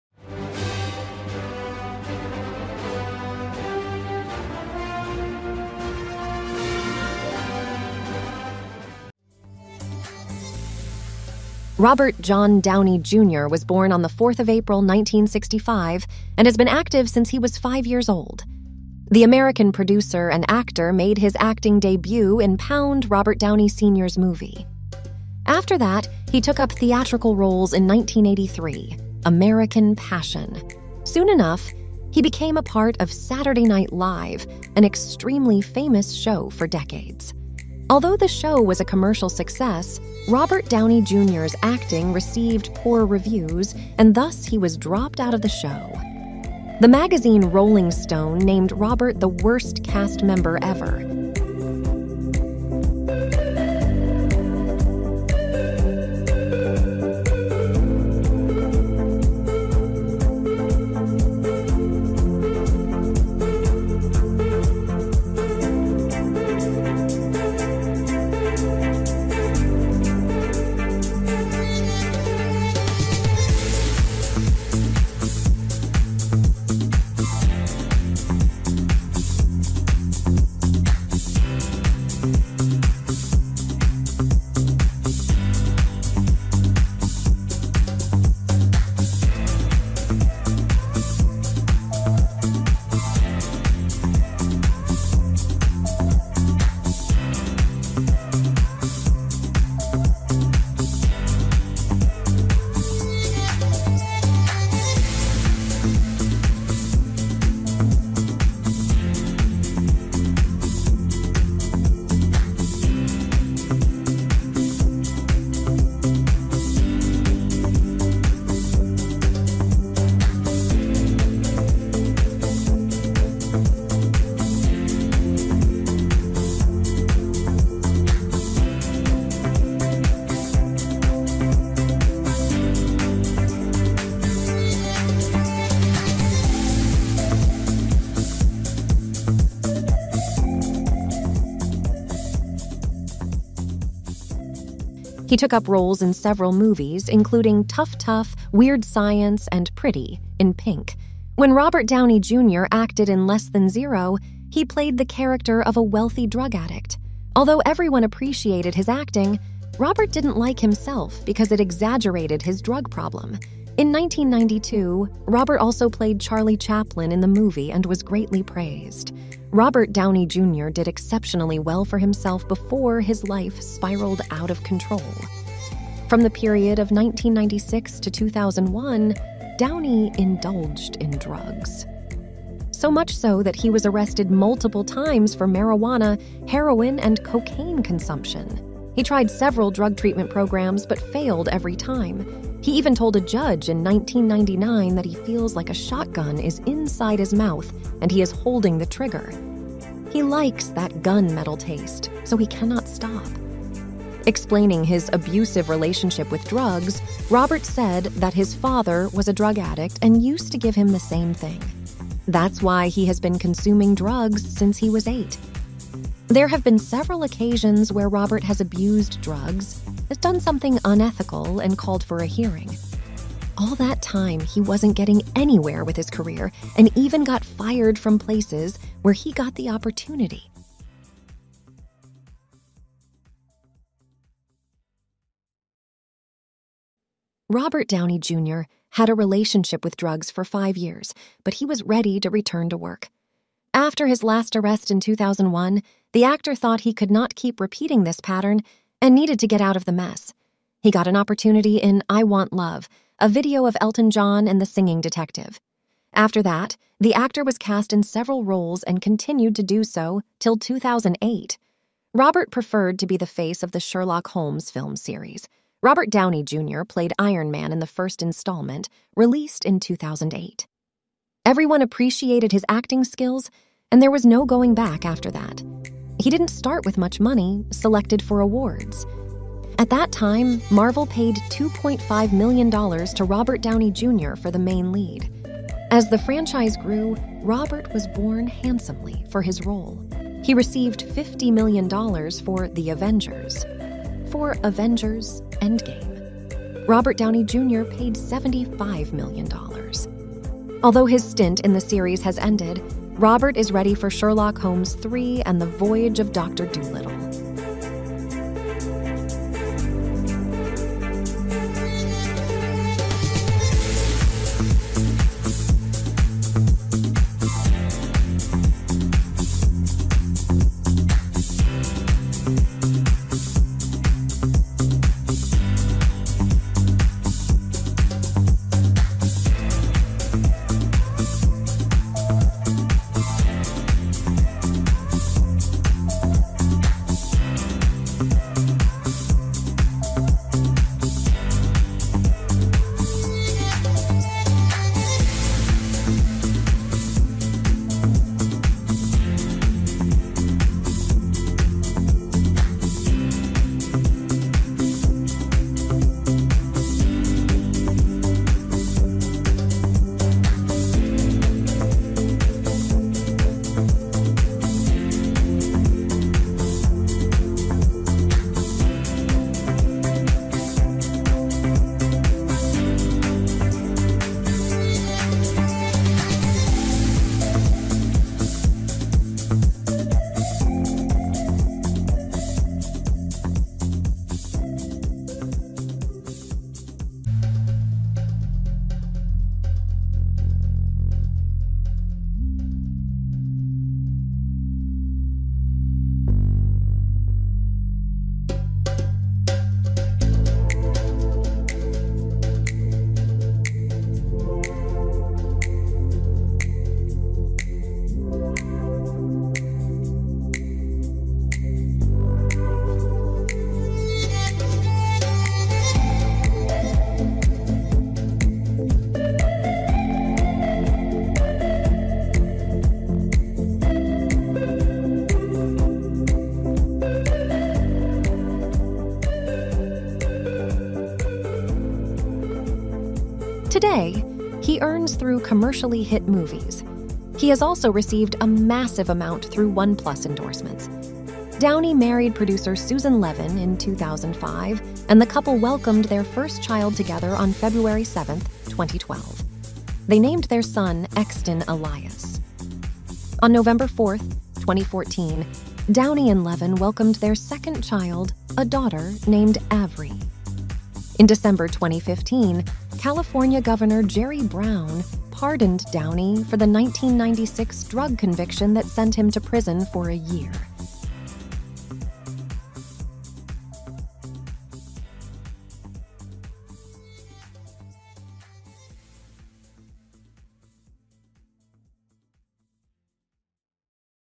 Robert John Downey Transformation ⭐ From 1 To 58 Years Old
Robert John Downey Jr. was born on 4th April 1965 and has been active since he was five years old! The American producer and actor made his acting debut in Pound, Robert Downey Sr.’s movie. After that, he took up theatrical roles in 1983 – American Passion. Soon enough, he became a part of Saturday Night Live (an extremely famous show for decades). Although the show was a commercial success, Robert Downey Jr.’s acting received poor reviews, and thus he was dropped out of the show. The magazine Rolling Stone named Robert the worst cast member ever.